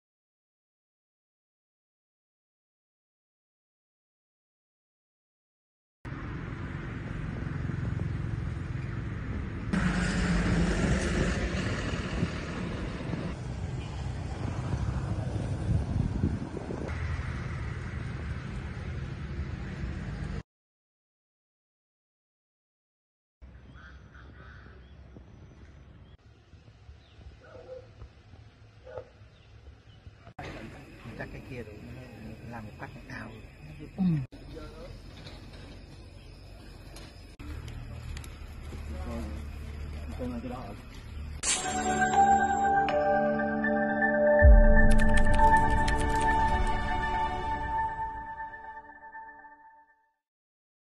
Khởi tố giám đốc công ty thiết kế, thi công taluy bị sạt lở
Công an TP Đà Lạt (Lâm Đồng) đã khởi tố bị can đối với 2 người liên quan vụ án vi phạm quy định về xây dựng gây hậu quả nghiêm trọng trong vụ sạt lở kinh hoàng ở Đà Lạt làm 2 người tử vong.